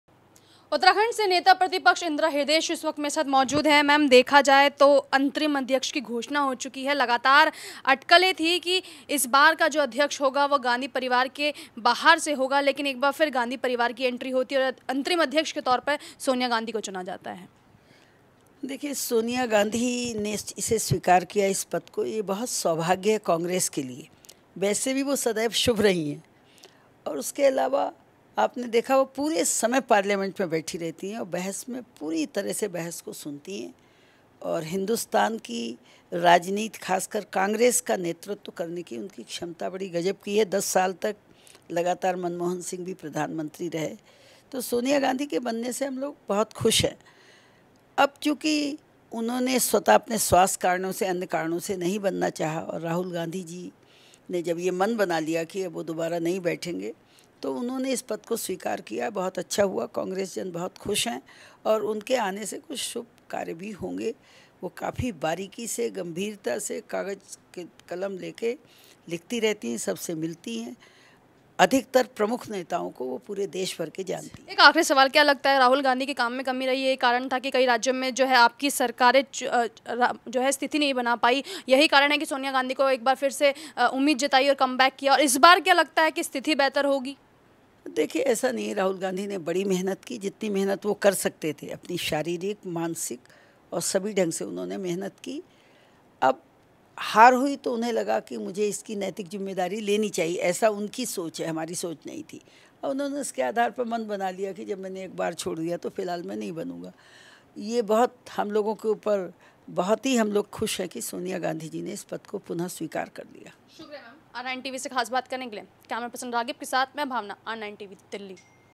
‘गांधी परिवार के कारण ही कांग्रेस में जोश और जज्बा पैदा होता है’
सोनिया गांधी के कांग्रेस के अंतरिम अध्यक्ष बनने पर उत्तारखंड में   प्रतिपक्ष की नेता इंदिरा हृदयेश ने  कहा कि नेता सोनिया गांधी का अंतरिम अध्यक्ष बनना कांग्रेस के लिए अच्छा है..